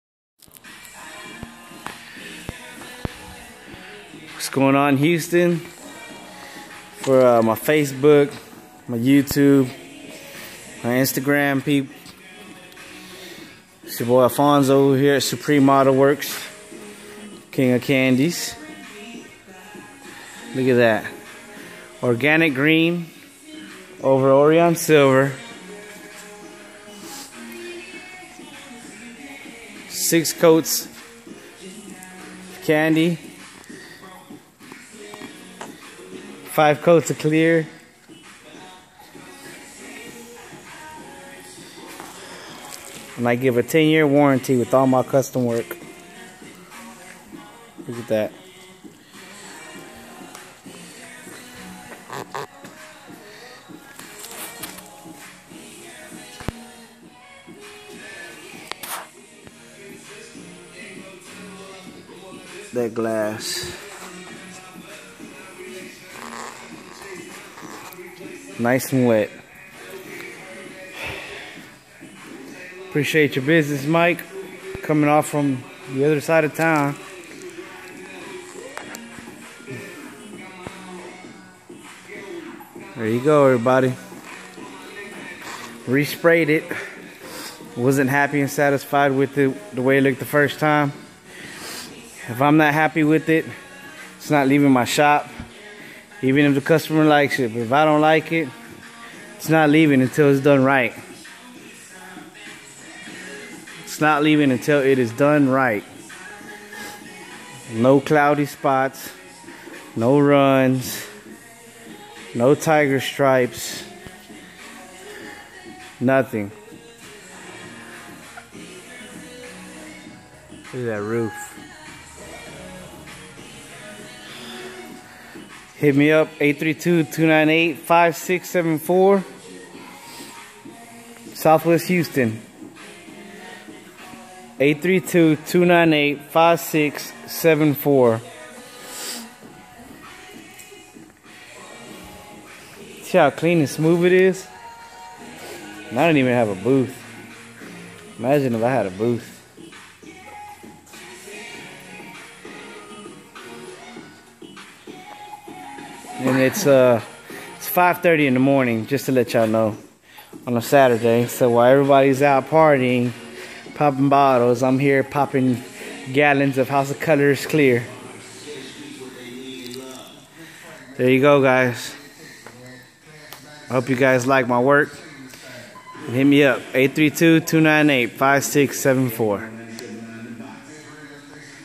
Supreme Autoworks Organic green H.O.K
Organic green over orion silver 6 coats of kandy 5 coats of clear!  2007 Buick Lucerne all HOK!